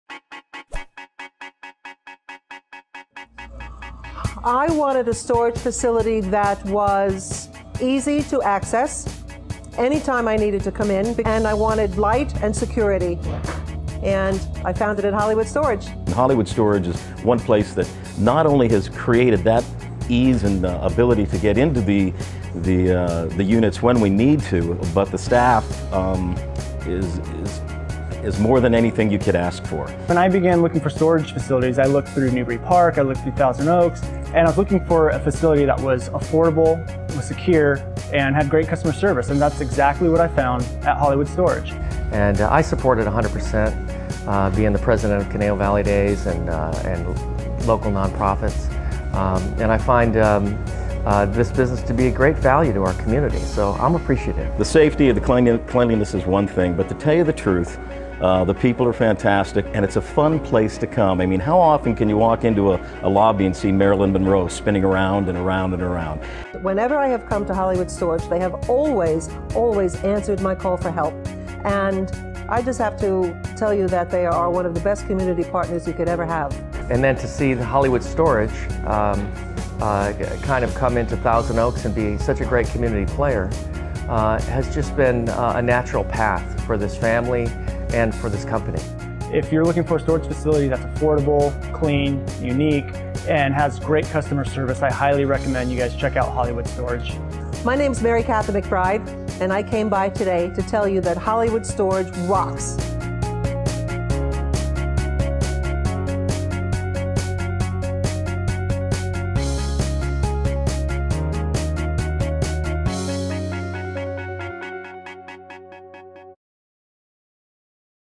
Hollywood Storage Center - Customer Reviews and Testimonials - Newbury Park, CA
Hollywood Storage Center in Newbury Park strives to offer the best customer service of any self-storage facility in Newbury Park and Thousand Oaks. This video shows the reviews of some of our customers which resulted from the affordable pricing and customer service we offer at Hollywood Storage. These are real reviews from real customers.

Hollywood Storage Newbury Park